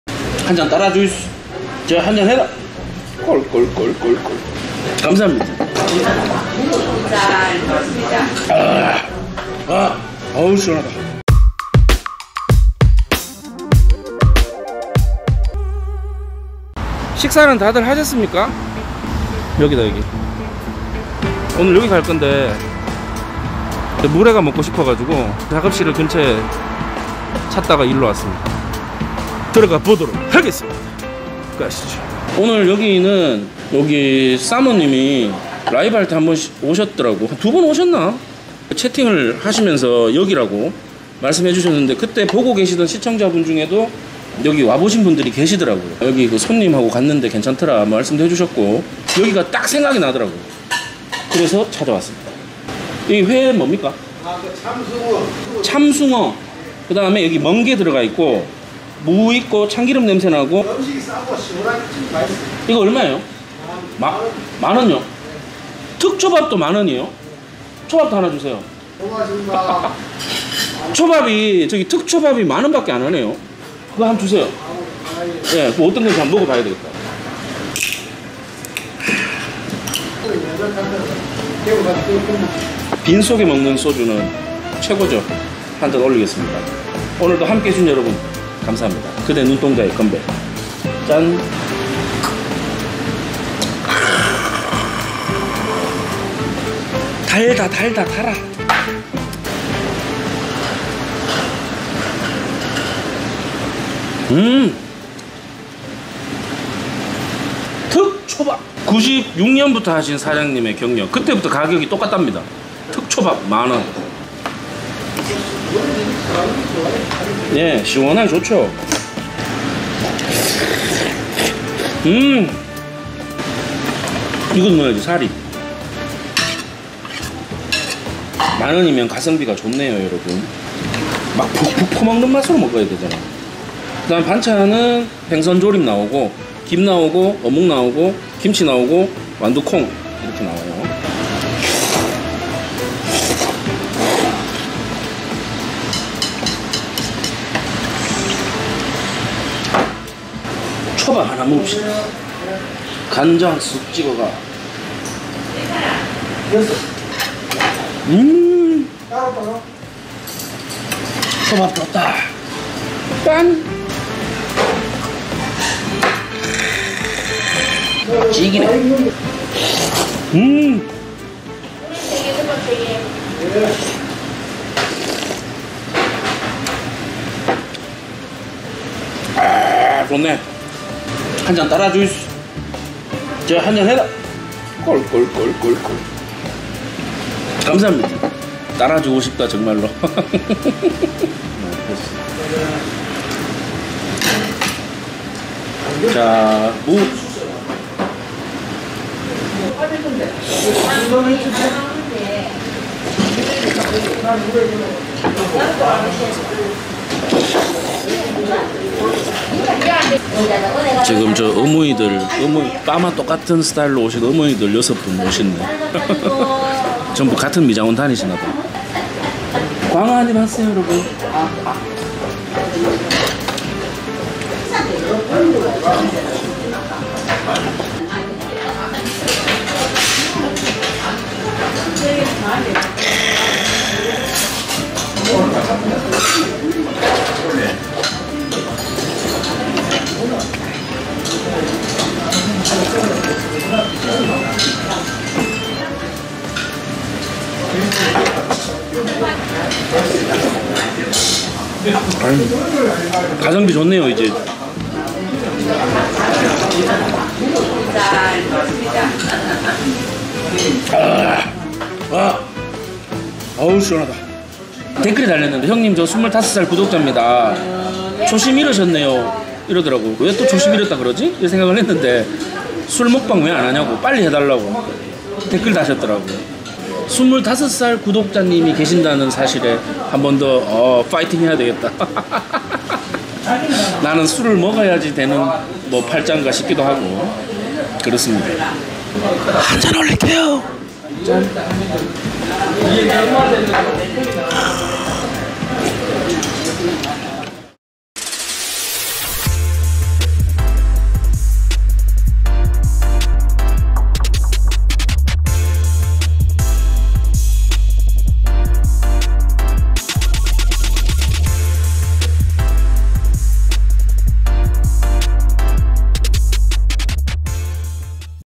물회 특초밥 얼마요? 가성비 횟집에서 낮술 한잔했습니다ㅣ심뽕티비
가는길-조선장회수산
대구 달서구 와룡로 7
매일 11:00 - 22:30
ㅡㅡㅡㅡㅡㅡㅡㅡㅡㅡㅡㅡㅡㅡㅡㅡㅡㅡㅡㅡㅡㅡㅡㅡ
제가 소개하는 음식점들이 제일 맛있고 최고다 그런거 아닙니다
이런집도 있구나하는 마음으로 봐주시면 감사하겠습니다^^

행사&강의&유튜브 문의